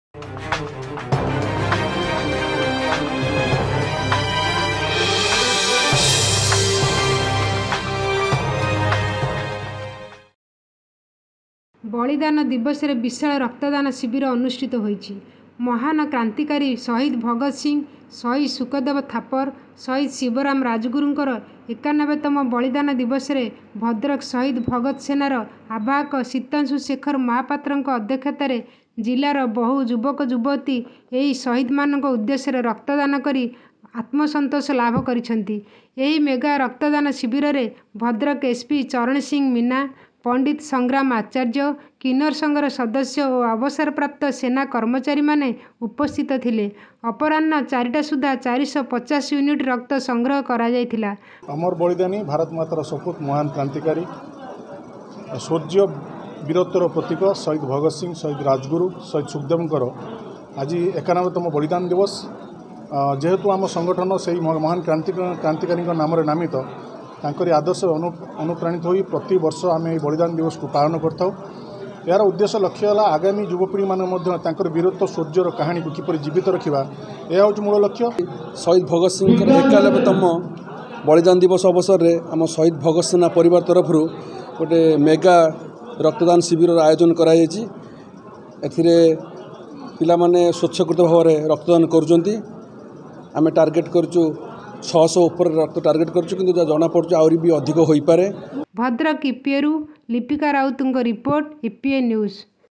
ବଳିଦାନ ଦିବସରେ ବିଶାଳ ରକ୍ତଦାନ ଶିବିର ଅନୁଷ୍ଠିତ a mega Blood Donation Camp Organised
ଭଦ୍ରକ : ତ୍ୟାଗ,ସମର୍ପଣ,ଶୌର୍ଯ୍ୟ,ବଳିଦାନର ପ୍ରତୀକ,କୋଟି କୋଟି ଯୁବକ ମାନଙ୍କ ହୃଦୟର ସ୍ପନ୍ଦନ,ଅଦିତ୍ୟ ସାହସ,ଭାରତ ମାତା ର ସୁଯୋଗ୍ୟ ସନ୍ତାନ,ମହାନ କ୍ରାନ୍ତିକାରୀ ଶହୀଦ୍ ଭଗତ୍ ସିଂ,ଶହୀଦ୍ ସୁଖଦେବ ଥାପର୍,ଶହୀଦ୍ ଶିବରାମ ରାଜଗୁରୁଙ୍କ ୯୧ ତମ ବଳିଦାନ ଦିବସରେ ତାଙ୍କର ସର୍ବୋଚ୍ଚ ବଳିଦାନ ସର୍ବଦା ସ୍ମରଣୀୟ ହୋଇ ରହିଛି ଆଜି ତାଙ୍କ ବଳିଦାନ ଦିବସରେ ଭଦ୍ରକ ସହିଦ ଭଗତ ସେନାର ଆବାହକ ସୀତାଂଶୁ ଶେଖର ମହାପାତ୍ର ଙ୍କ ଅଧକ୍ଷତାରେ ଜିଲ୍ଲାର ଅନେକ ଯୁବକ ସ୍ୱତଃ ପ୍ରବୃର୍ତ୍ତ ଭାବେ ଆସି ମହାରାଜା ଅଗ୍ରସେନ ଭବନ ଠାରେ ନିଜର ରକ୍ତଦାନ କରି ଖୁସି ବ୍ୟକ୍ତ କରିଛନ୍ତି |ଏହି ବିଶାଳ ରକ୍ତଦାନ ଦିବସ ଅବସରରେ ଭଦ୍ରକ ଆରକ୍ଷୀ ଅଧିକ୍ଷକ ଚରଣ ସିଂ ମିନା, ପଣ୍ଡିତ ସଂଗ୍ରାମ ଆଚାର୍ଯ୍ୟ, କିନ୍ନର ସଂଘର ସଦସ୍ୟ, ଅବସରପ୍ରାପ୍ତ ସେନା କର୍ମଚାରୀଙ୍କ ସହ ସହିଦ ଭଗତ ସେନାର ଶତାଧିକ କର୍ମକର୍ତା ମାନେ ଜୋଗଦେଇଥିଲେ |ଏହି ଦିବସରେ ଅପରାହ୍ନ 4 ଟା ସୁଦ୍ଧା 450 ୟୁନିଟ ରକ୍ତ ସଂଗ୍ରହ କରାଯାଇଥିବା ବେଳେ ଏଭଳି ଏକ ମହତ କାର୍ଯ୍ୟକ୍ରମକୁ ନିମନ୍ତ୍ରିତ ଅତିଥି ମାନେ ପ୍ରଶଂସା କରିବା ସହ ଅନ୍ୟ ଜଣଙ୍କ ଜୀବନ ବଞ୍ଚାଇବା ପାଇଁ ଯେଉଁ ମାନେ ନିଜ ରକ୍ତ ବିନ୍ଦୁ କୁ ସ୍ଵ ଇଛାରେ ପ୍ରଦାନ କରୁଛନ୍ତି ସେମାନଙ୍କୁ  ସତକୋଟି ପ୍ରଣାମ କରିବା ସହ ଅନ୍ତରରୁ ଧନ୍ୟବାଦ ଜଣାଇଛନ୍ତି ଆବାହକ ସୀତାଂଶୁ ଶେଖର ମହାପାତ୍ର |